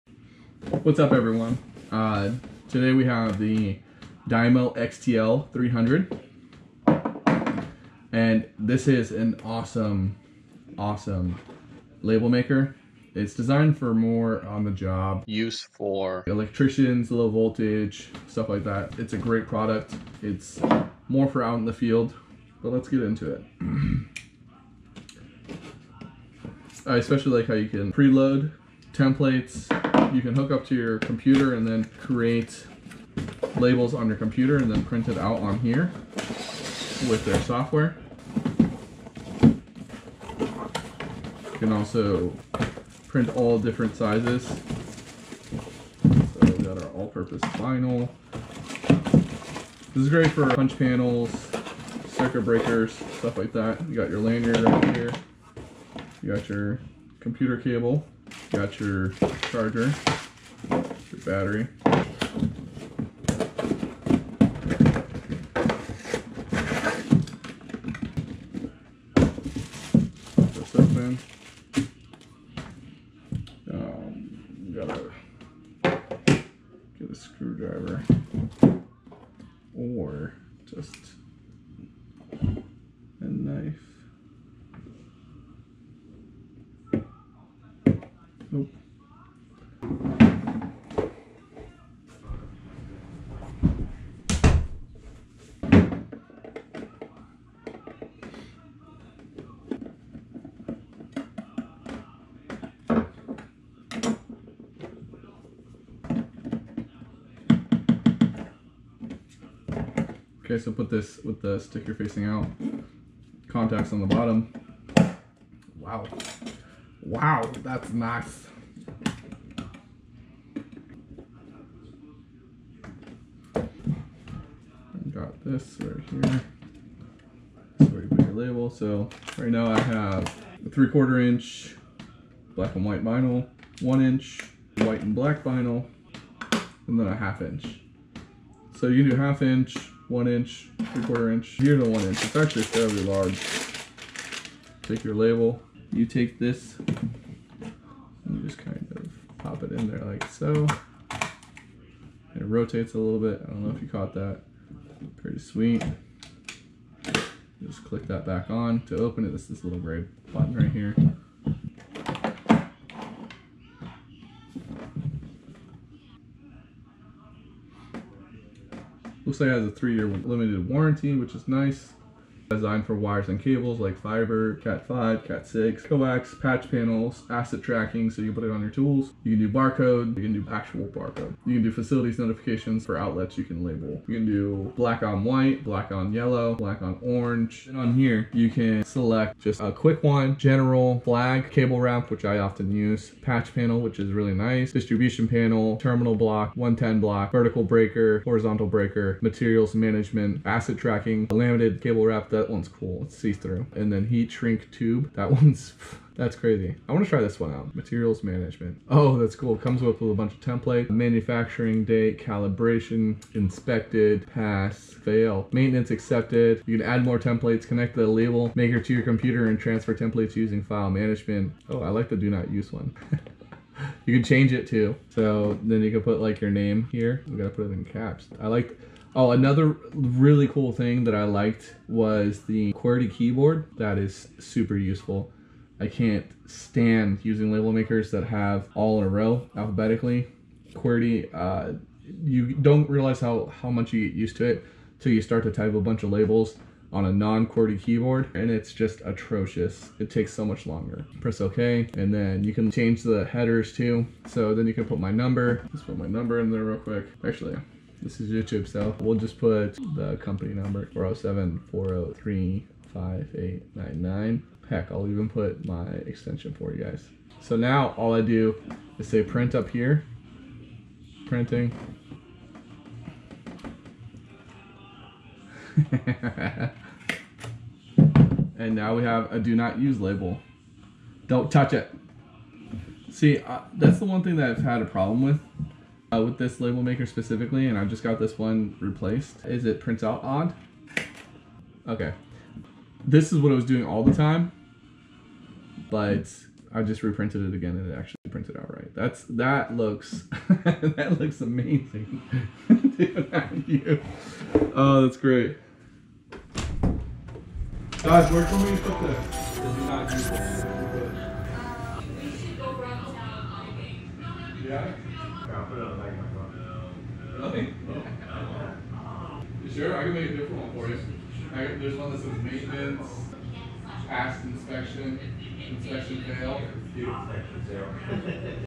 DYMO XTL 300 Unboxing Video & First Use
Today we unbox the Dymo XTL 300 Label Maker!